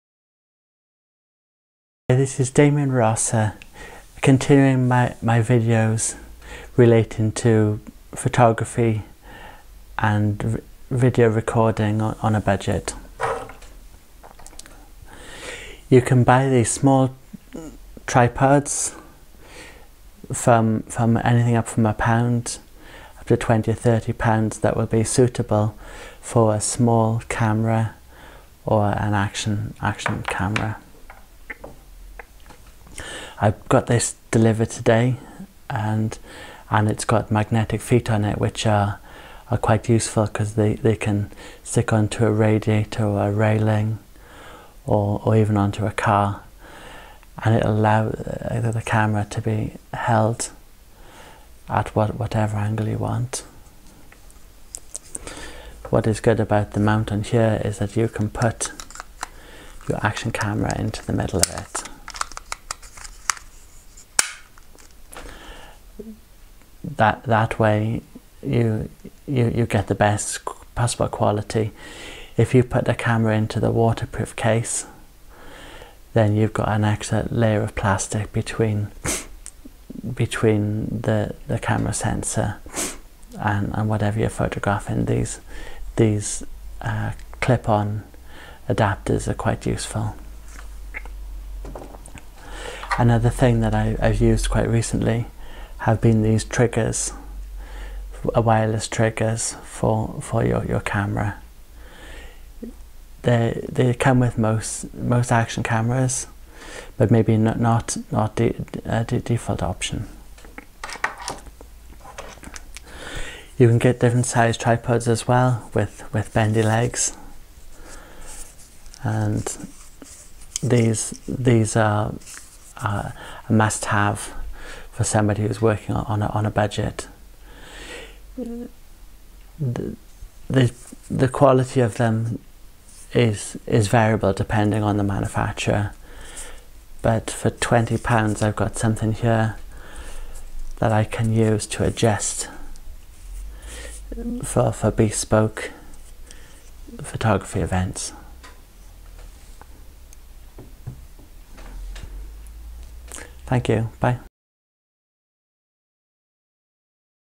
Simple tripods to improve photos and video
Presenting simple tripods, that can be used to make video and photography easier.